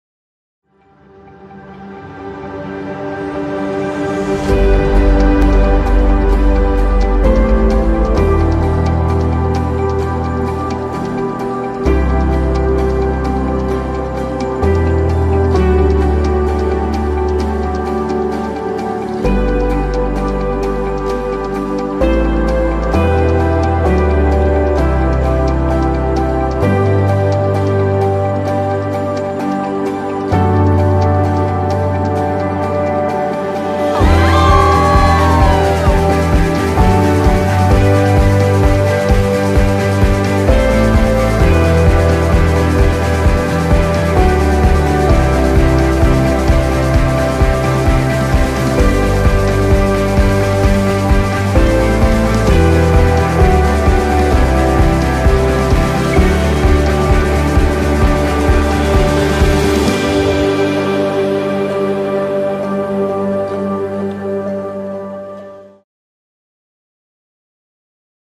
Sony + Simon wedding trailer at W hotel Koh Samui
Bride & Groom : Sony & Simon
Wedding Venue : W Hotel Koh Samui
wedding film by :nokatoa